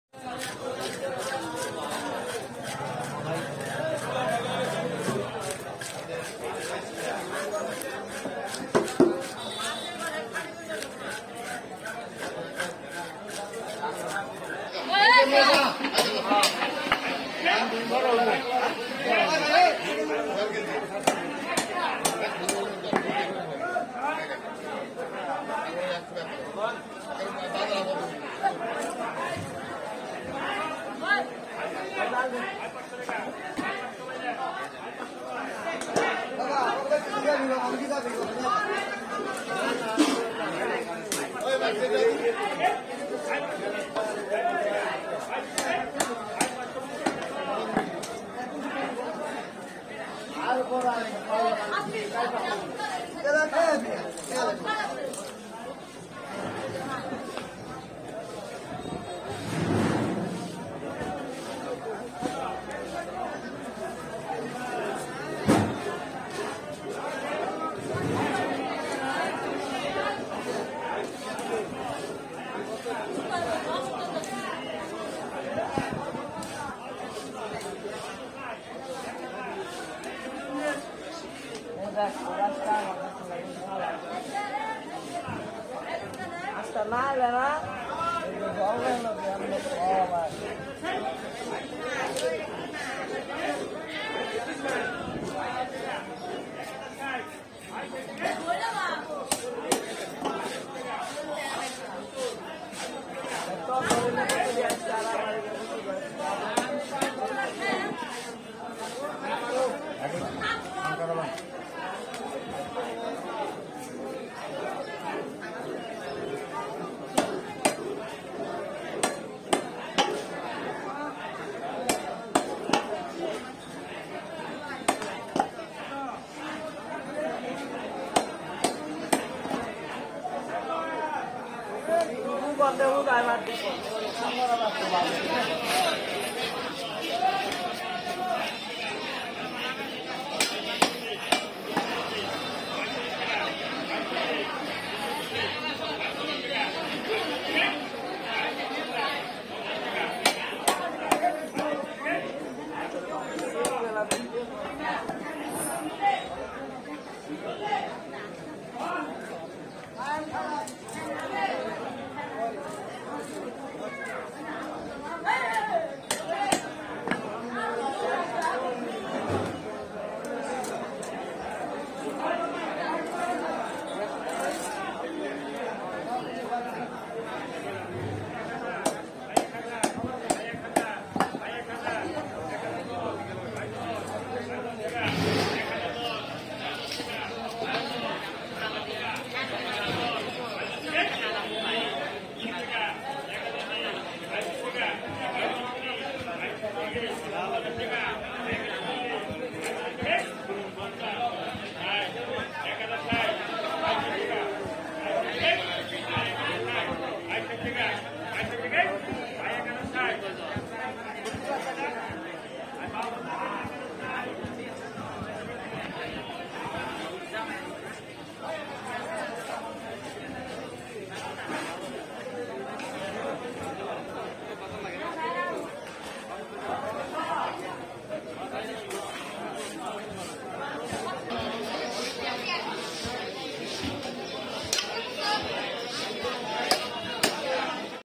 Amazing Big Rohu | Fish Cutting skills in Bangladesh fish market
Amazing Cutting Skills | Big Barracuda Fish Fish Cutting Live In Fish Market

আসসালামু আলাইকুম আরএম চ্যানেলের পক্ষ থেকে শুভেচ্ছা আমাদের এই চ্যানেলে বড় মাছ কাটার ভিডিও দেখানো হয়👇
fish cutting